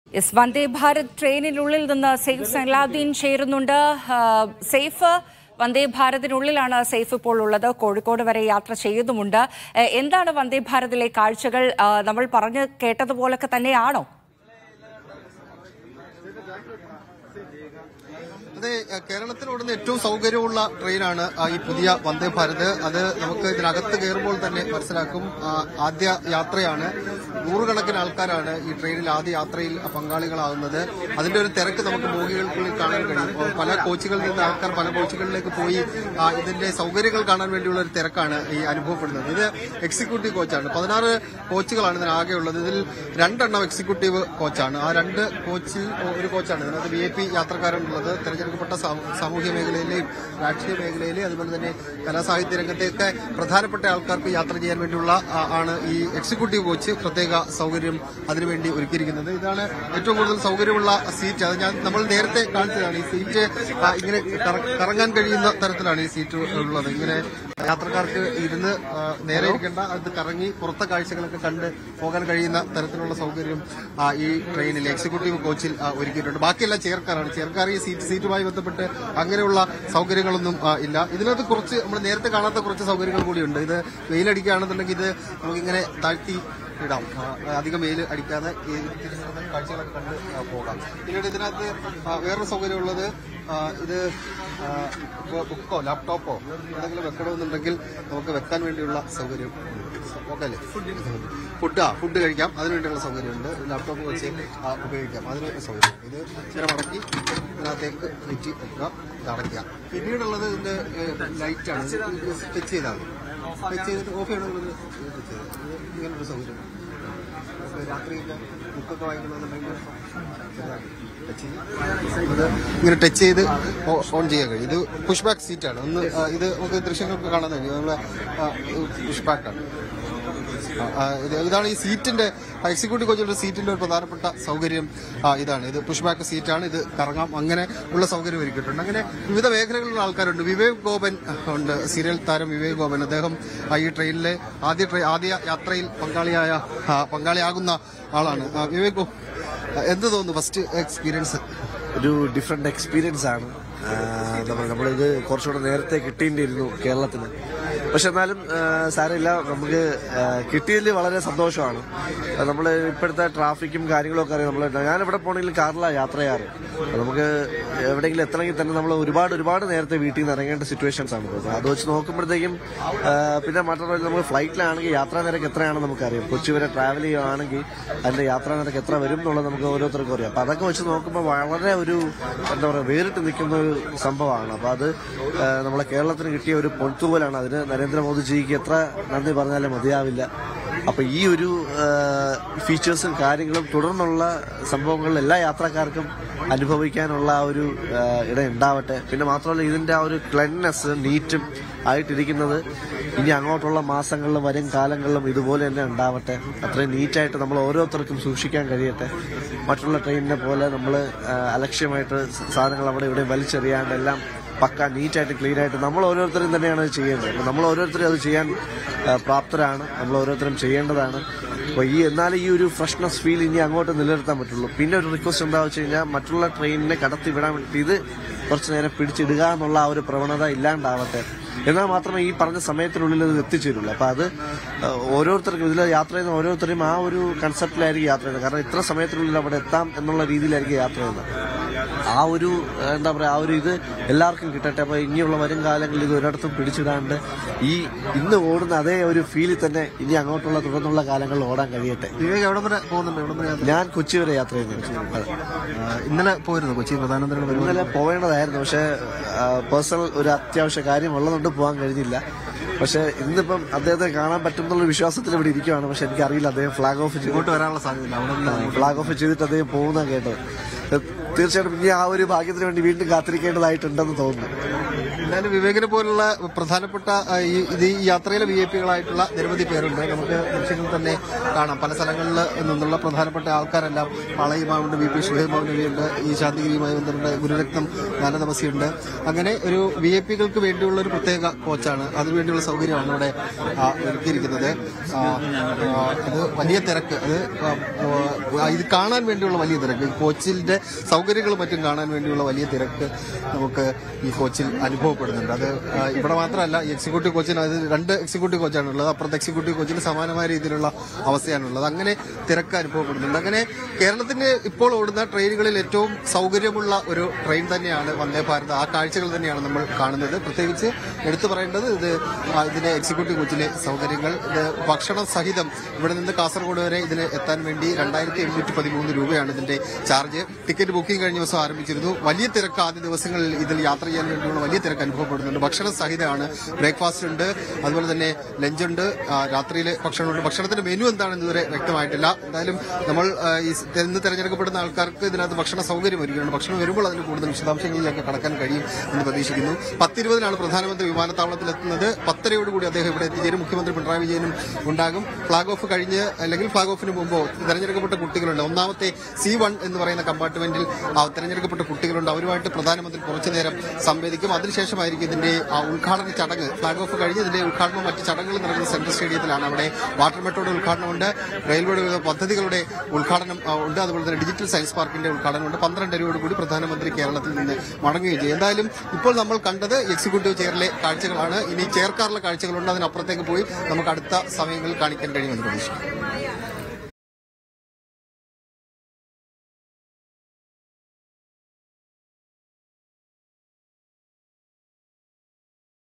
"കേരളത്തിന് കിട്ടിയ പൊൻതൂവൽ, പ്രധാനമന്ത്രിക്ക് എത്ര നന്ദി പറഞ്ഞാലും മതിയാവില്ല"
People's experience on Vande Bharat